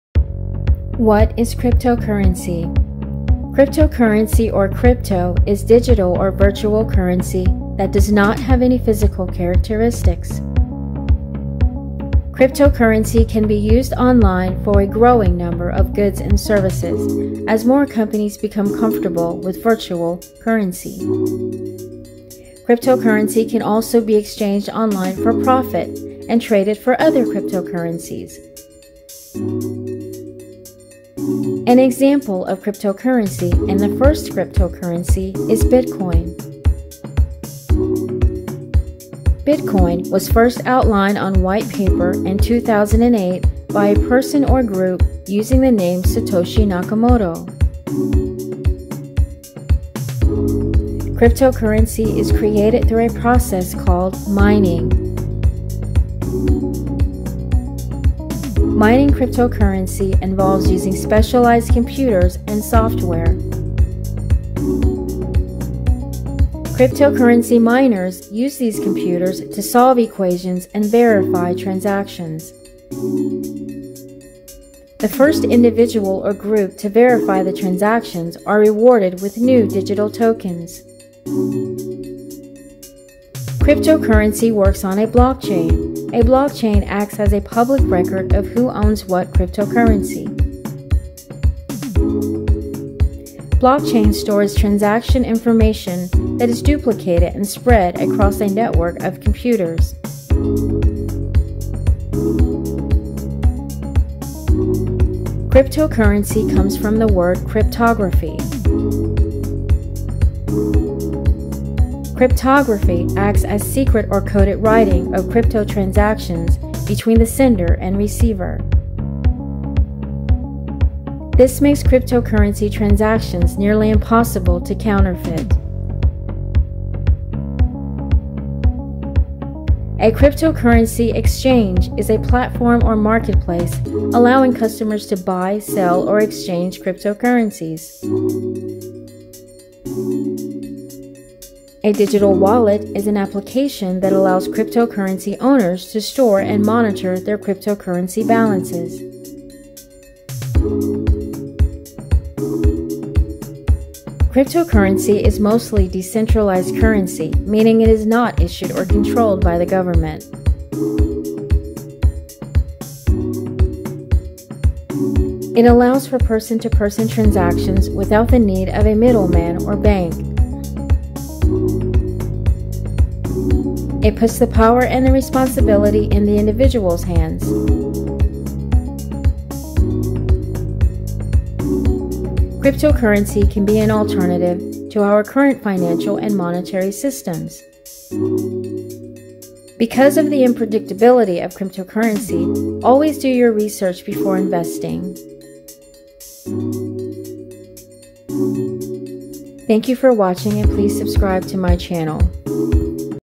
What is Cryptocurrency? Cryptocurrency Explained for Beginners in 3 minutes
In this video Cryptocurrency explained for beginners. How cryptocurrency works. Crypto basics
Cryptocurrency Explained for Beginners